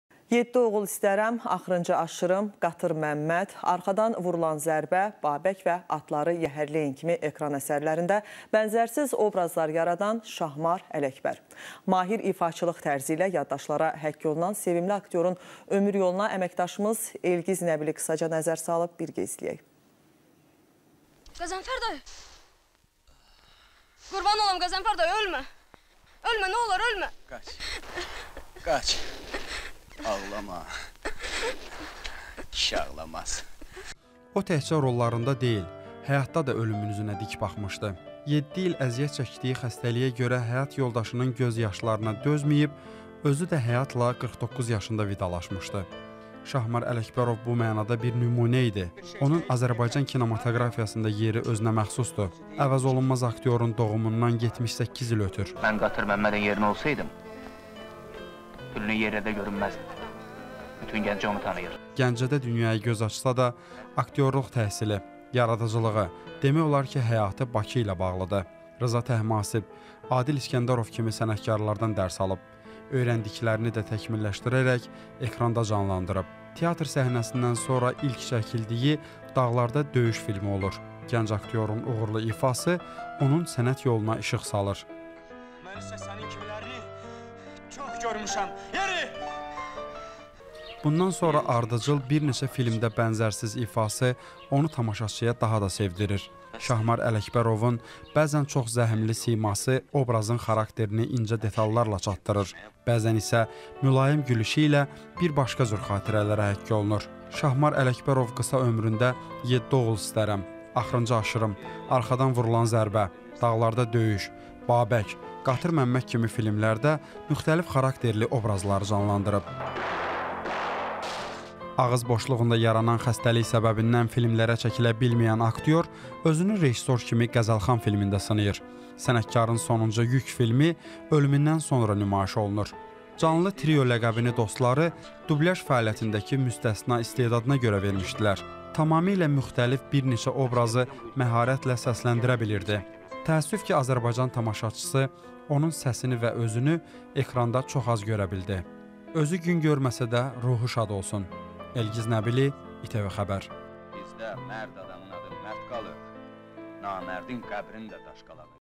"Canlı Trio" ləqəbli Şahmar Ələkbərovun ölüm səbəbi nə idi?
Şahmar Ələkbərovun anadan olmasından 78 il ötür

O təkcə rollarında deyil, həyatda da ölümün üzünə dik baxmışdı. 7 il əziyyət çəkdiyi xəstəliyə görə həyat yoldaşının göz yaşlarına dözməyib özü də həyatla 49 yaşında vidalaşmışdı. Şahmar Ələkbərov bu mənada bir nümunə idi. Onun Azərbaycan kinematoqrafiyasında yeri özünəməxsusdur. Əvəzolunmaz aktyorun doğumundan 78 il ötür. Gəncədə dünyaya göz açsa da aktyorluq təhsili, yaradıcılığı, demək olar həyatı Bakı ilə bağlıdır. Rza Təhmasib, Adil İsgəndərov kimi sənətkarlardan dərs alıb. Öyrəndiklərini də təkmilləşdirərək ekranda canlandırıb. Teatr səhnəsindən sonra ilk çəkildiyi Dağlarda döyüş filmi olur. Gənc aktyorun uğurlu ifası onun sənət yoluna işıq salır…
Bundan sonra ardıcıl bir neçə filmdə bənzərsiz ifaası onu tamaşaçıya daha da sevdirir. Şahmar Ələkbərovun bəzən çox zəhmli siması obrazın xarakterini incə detallarla çatdırır, bəzən isə mülayim gülüşü ilə bir başqa cür xatirələrə həkk olunub.
Şahmar Ələkbərov qısa ömründə “Yeddi oğul istərəm", "Axırıncı aşırım", "Arxadan vurulan zərbə”, Dağalarda döyüş”, Babək, Qatır Məmməd kimi fimlərdə müxtəlif xarakterli obrazları canlandlrlb. Ağız boşluğunda yaranan problemlərdən dolayı filmlərə çəkilən bilməyən aktyor özünü rejissor kimi Qəzəlxan filmində sınayır. Sənətkarın sonuncu Yük filmi ölümündən sonra nümayiş olunub. "Canlı Trio" ləqəbini dostları dublyaj fəaliyyətindəki müstəsna istedadına görə vermişdilər. Tamamilə müxtəlif obrazları məharətlə səsləndirə bilirdi. Təəssüf ki Azərbaycan tamaşaçısı onun səsini və özünü ekranda çox az görə bildi.
Elgiz Nəbili,  İTV XƏBƏR